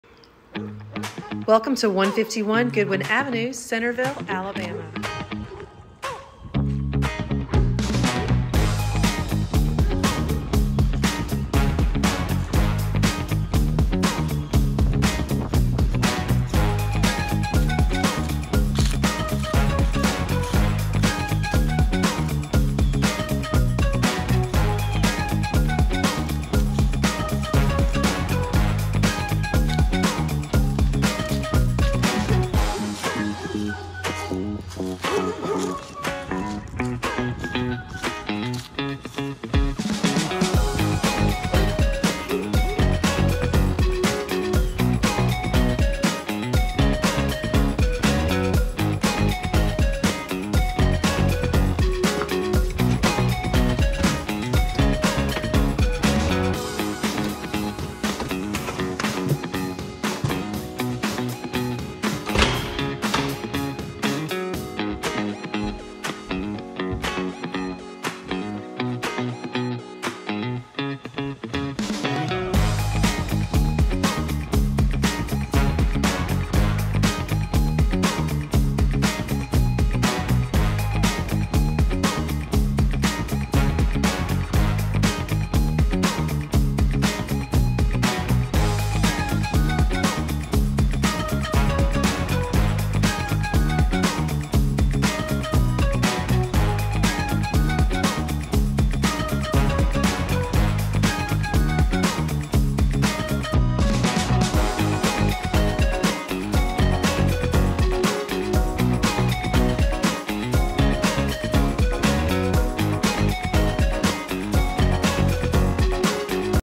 151 Goodwin Avenue VT
CURRENTLY OFF MARKET Quick virtual tour of this lovely 3/2 on quiet street in Centreville. Come see! Bring a friend!